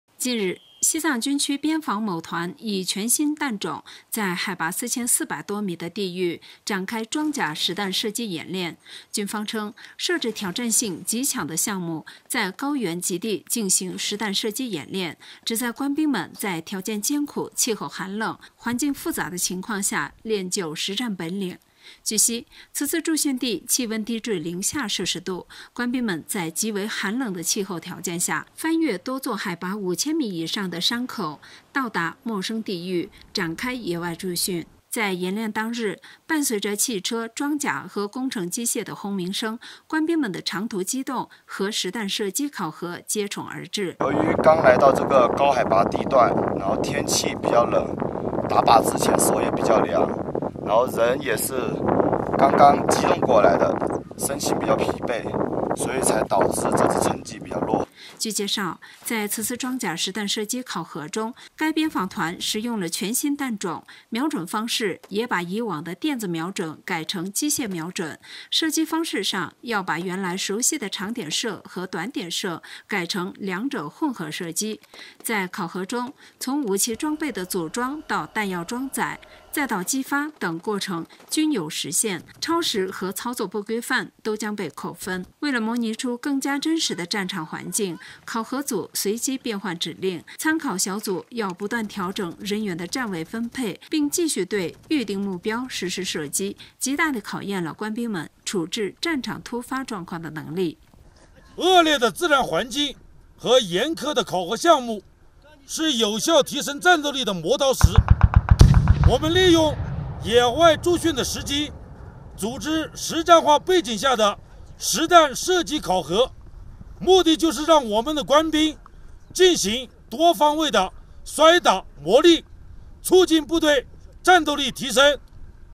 西藏军区边防某团以全新弹种展开装甲实弹射击演练
近日，西藏军区边防某团以全新弹种，在海拔接近4400多米的地域，展开装甲实弹射击演练。军方称，设置挑战性极强的项目，在高原“极地”进行实弹射击演练，旨在官兵们能在条件艰苦、气候寒冷、环境复杂的情况下，练就实战本领。